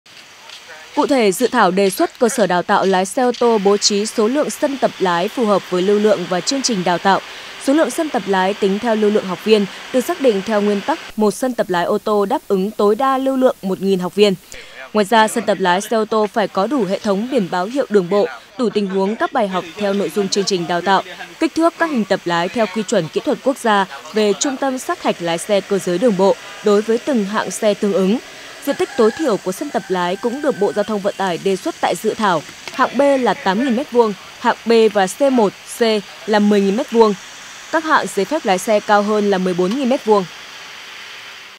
Mỗi sân dạy lái đáp ứng yêu cầu tới 1.000 học viên | Tin tức
Mỗi sân dạy lái xe ô tô phải đáp ứng yêu cầu cho 1.000 học viên. Đây là nội dung đề xuất trong Dự thảo Nghị định quy định về hoạt động đào tạo và sát hạch lái xe do Bộ GTVT chủ trì soạn thảo. Hiện, Bộ Tư pháp đang được tổ chức thẩm định Dự thảo này.